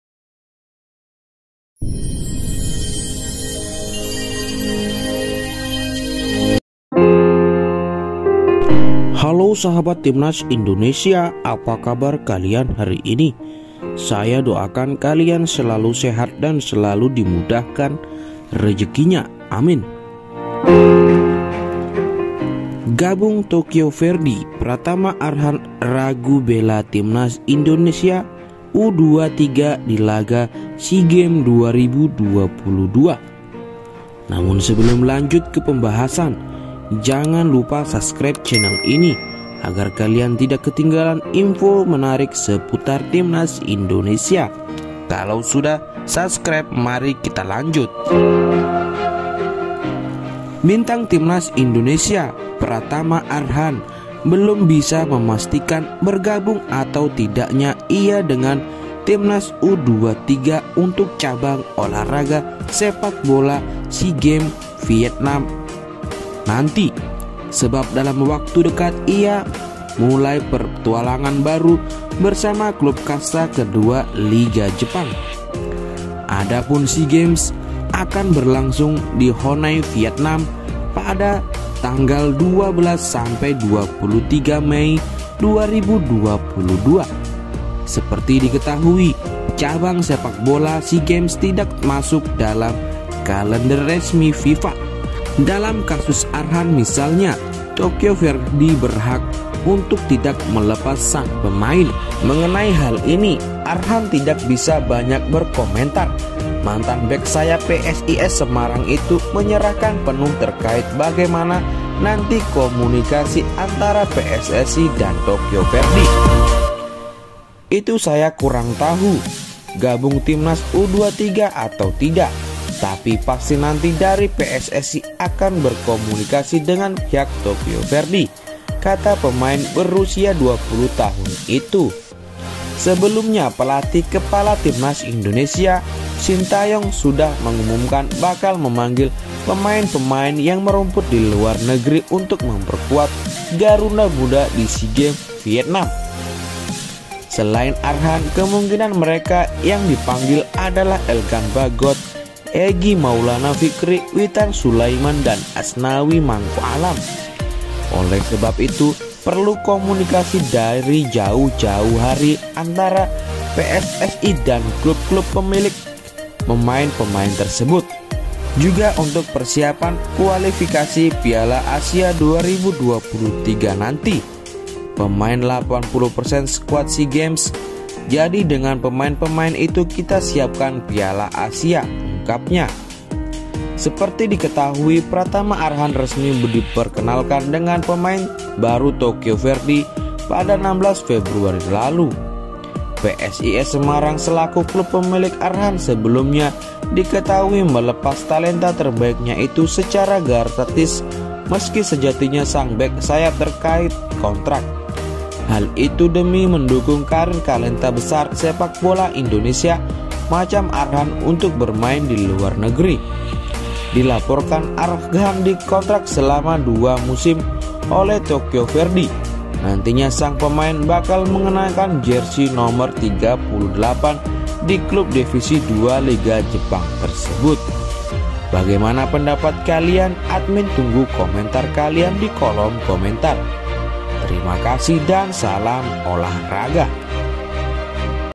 Terungkap.! ini Penyebab Pratama Arhan diragukan Tampil & bela Timnas IndonesiaU22 di SEA Games 2022
SEA Games 2021 tidak masuk kalender FIFA sehingga klub, termasuk Tokyo Verdy tidak wajib melepas pemainnya. Praktis, kans Pratama Arhan gabung Timnas Indonesia U-22 cukup kecil.